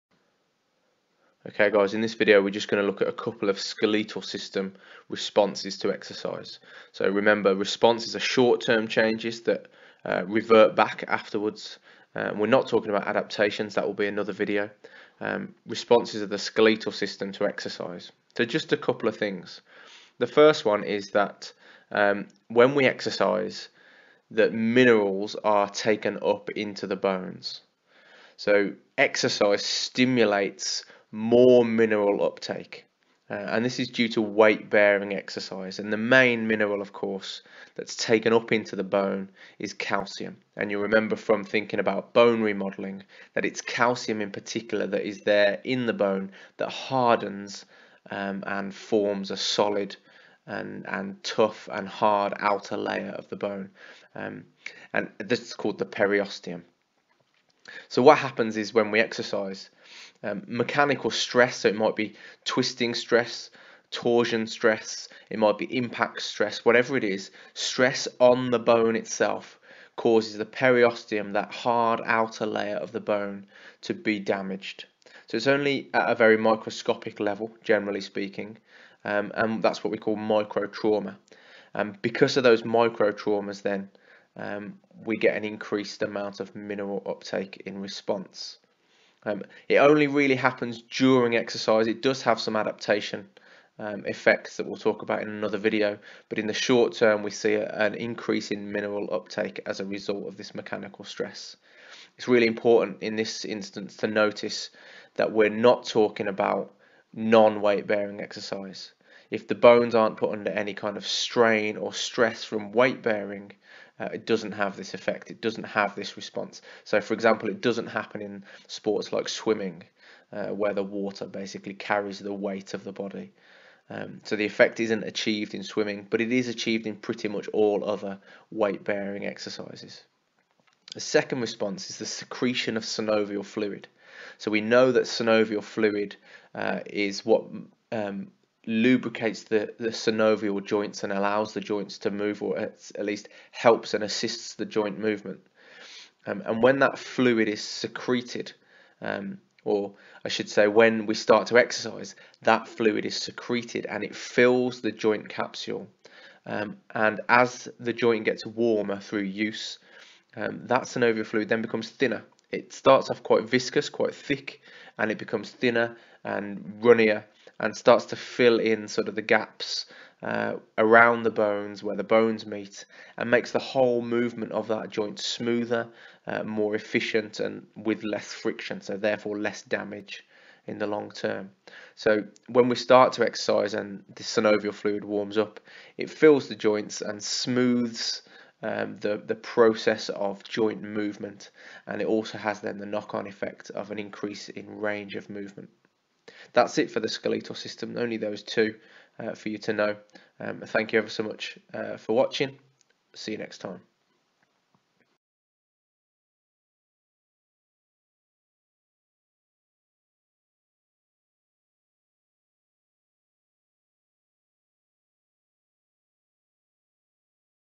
Responses to Exercise | Skeletal System 06 | Anatomy & Physiology
BTEC Level 3 Nationals in Sport (from 2016)
Unit 1: Anatomy & Physiology
A The effects of exercise and sports performance on the skeletal system
A4 Responses of the skeletal system to a single sport or exercise session